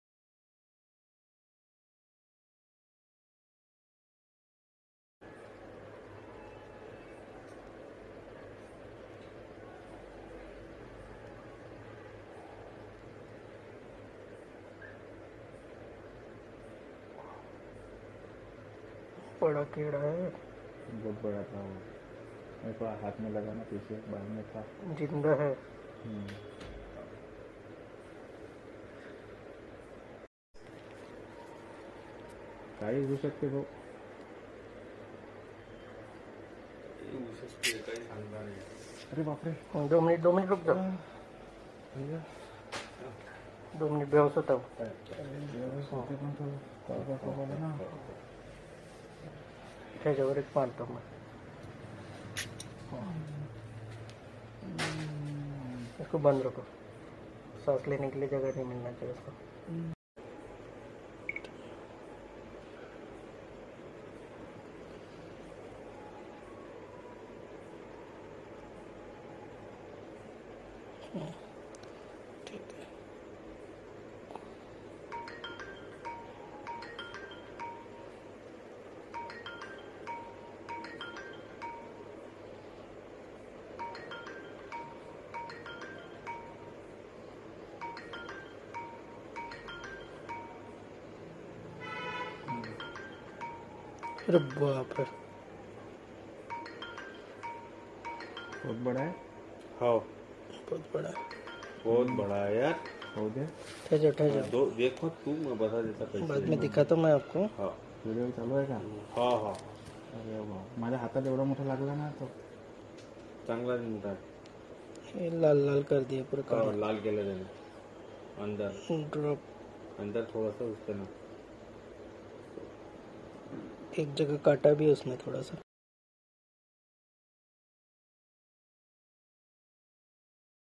Alive Flying Insect Cricket Removal From Ear
This video show endoscopic ear foreign body ( Alive Flying Insect - Cricket ) removal from ear in young man .
He came with history of flying insect stuck inside his ear few minutes back .
On endoscopic examination , alive black cricket seen inside his ear . Local anesthesia spray used to sedate insect . Later it is removed under endoscopic vision with the help of ear forcep .

Thanks .
GOOD DAY !!!